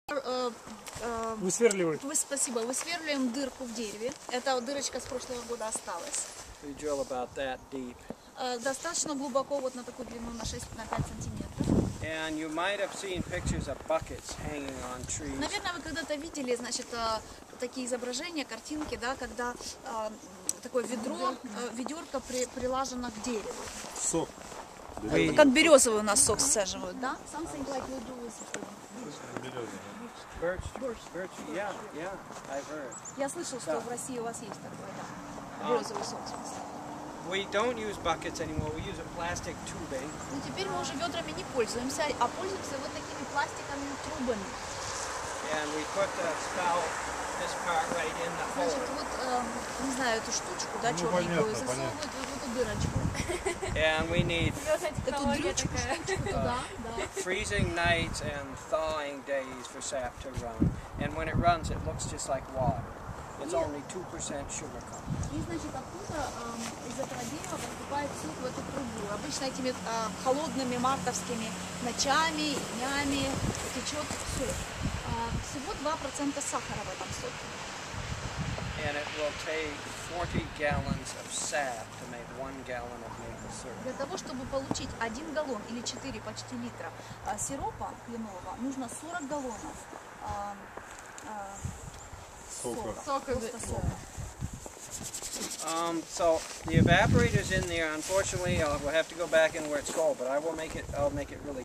Maple Syrup Diplomacy--Yaroslavl Visitors at Morse Farm Sugarworks
From December 3-11, 2010, the Vermont Council on World Affairs (VCWA) was proud to host an Open World delegation from Burlington's Sister City in Russia, Yaroslavl. The focus of this program was on 'accountable governance' and featured four officials from Yaroslavl's local and regional government, as well as a bilingual facilitator.

While in Vermont, this group had a chance to meet with business leaders, nonprofit organizations, and a variety of government officials at the national, state, and local levels--including Senator Bernie Sanders and Governor James Douglas.
In this clip Burr Morse, the proprietor of Morse Farm Sugarworks and one of the state's folk icons visits with our Russian visitors and explains the process of making maple syrup.  Seemingly every time we're in Montpelier with our international visitor groups, we stop by Morse Farms for a sampling and some shopping--thanks again to Burr and the rest of the staff for welcoming our visitors and sharing one of Vermont's finest traditions with us.